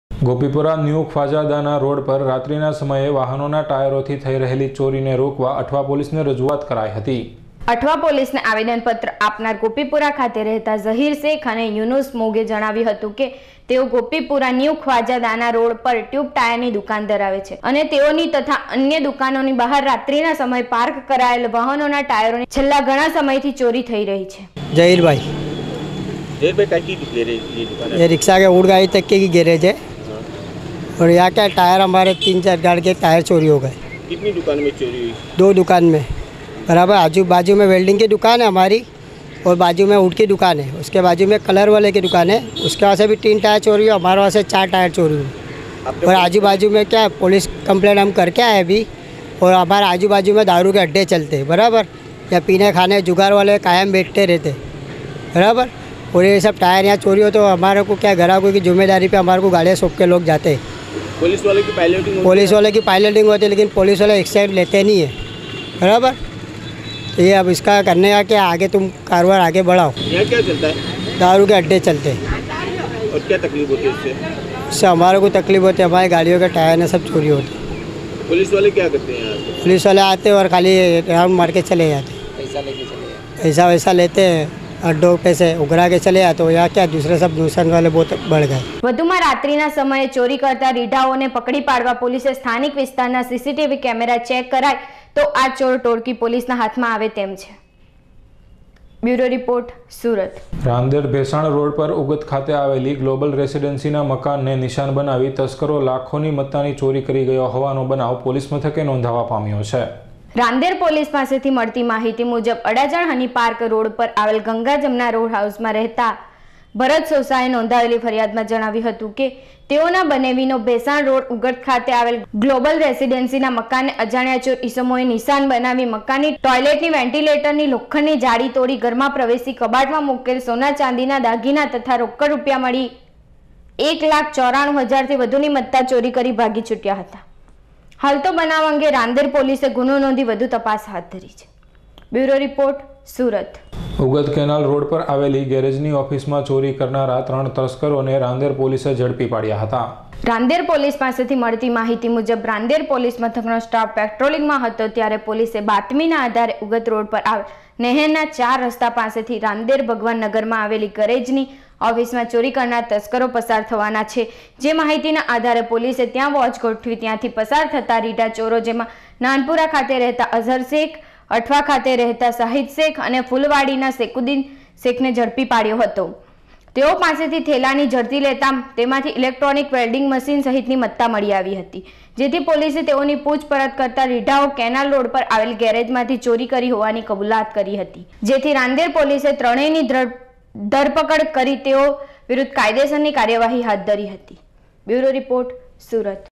Surat : ગોપીપુરામાં વાહનોના ટાયરોની થઈ રહેલી ચોરી
Surat : રાંદેર ભેસાણ પર રેસીડેન્સીના મકાનને નિશાન બનાવતા તસ્કરો
Surat : ઉગત કેનાલ રોડ પર ગેરેજની ઓફિસમાં ચોરી કરનારા ઝડપાયા